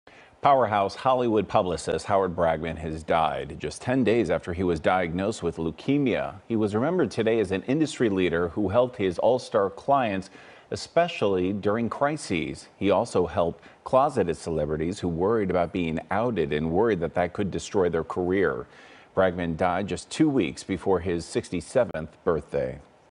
Well-known Hollywood publicist Howard Bragman dies at 66
Howard Bragman, a beloved Hollywood publicist who specialized in crisis relations and whose clients have included Monica Lewinsky, Cameron Diaz, Ricki Lake, Sharon Osbourne and Chaz Bono, has died after a short battle with leukemia. He was 66.